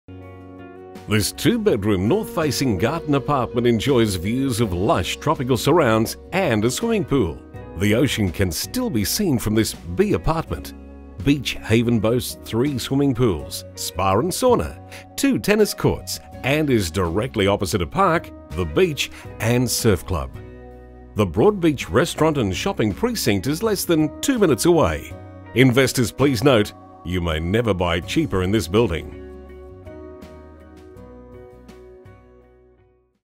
2B Beach Haven, 1 Albert Avenue, Broadbeach QLD By Rob Astbury
Property Video shoot of 2B Beach Haven, 1 Albert Avenue, Broadbeach QLD by PlatinumHD for Ray White Broadbeach